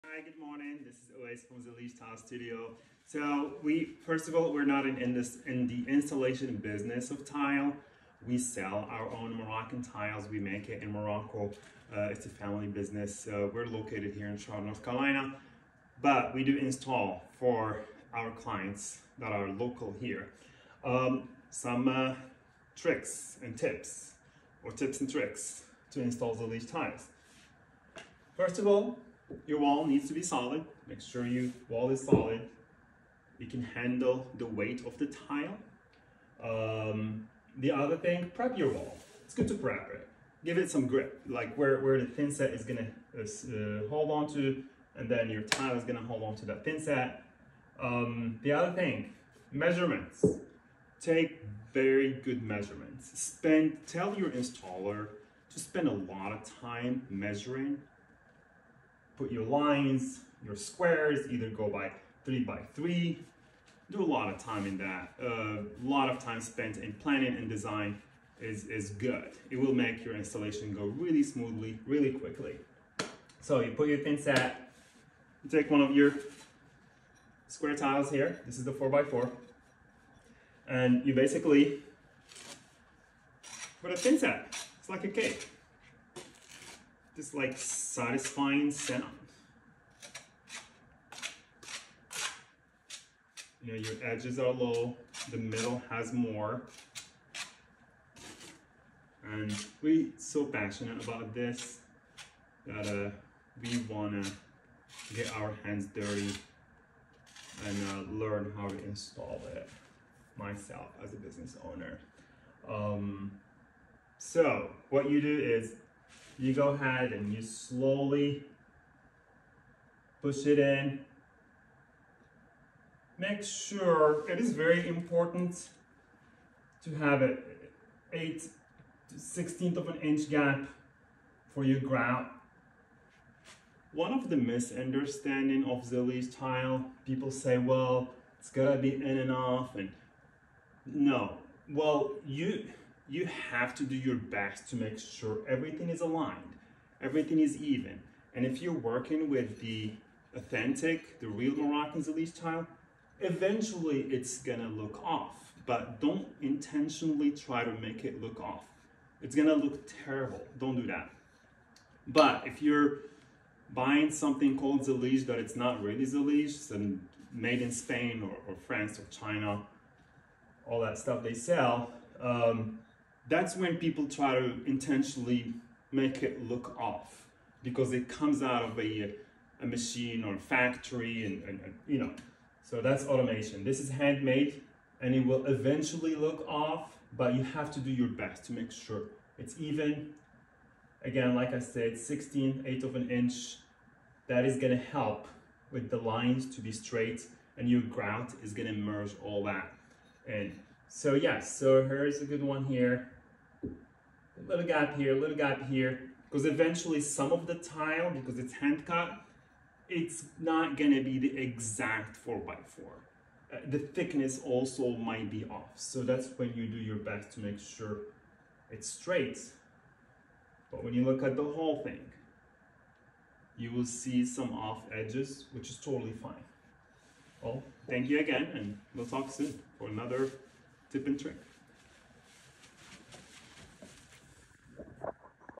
Zellige Tile Installation - Tips & Tricks
At Zellige Tile Studio we care about our clients and followers, we provide them with tips on how to install our Zellige Tile. This short video is about tips for installing 4x4 Zellige tile squares.

These Tips are for our Zellige Tile product only that customers buy from us, we do not take responsibility of any other tile that's not purchased from us directly. these are just tips and we give our clients detailed How To steps once they purchase Authentic Moroccan Zellige Tile.

We encourage you to hire a professional tile installer that has experience with Zellige.